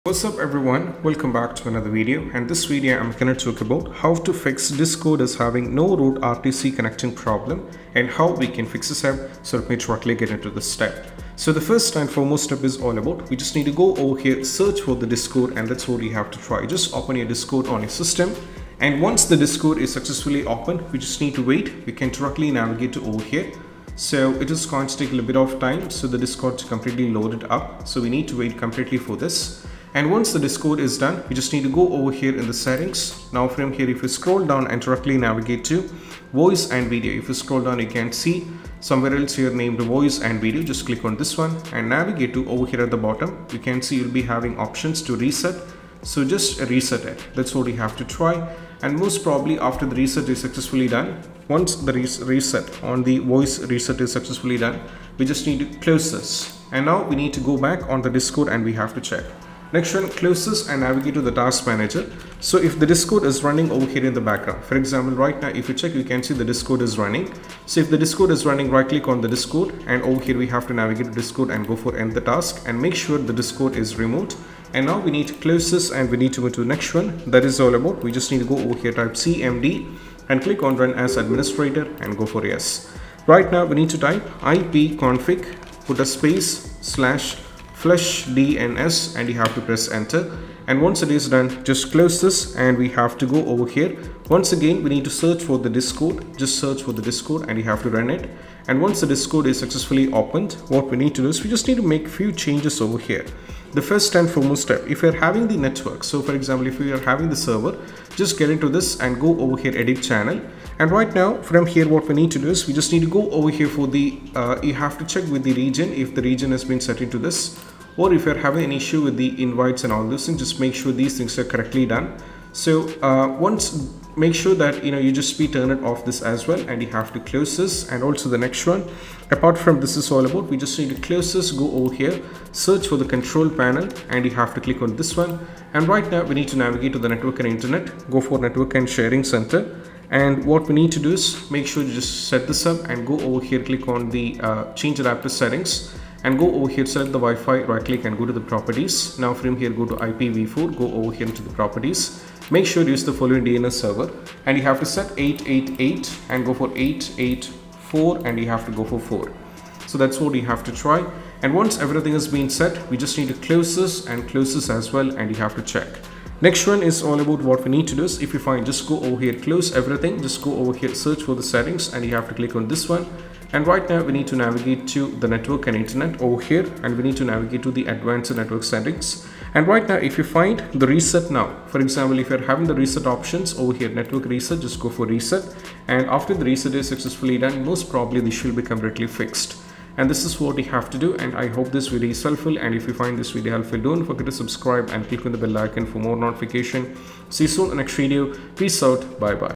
How to Fix Discord “No Route” or “RTC Connecting” Error
Learn how to fix the “No Route” or “RTC Connecting” error in Discord with this easy tutorial. If you’re having trouble with voice chat, we’ll walk you through simple steps to resolve it. From network restrictions and app settings to VPN conflicts, we’ll cover all the common causes and how to fix them so you can get back to chatting without any issues.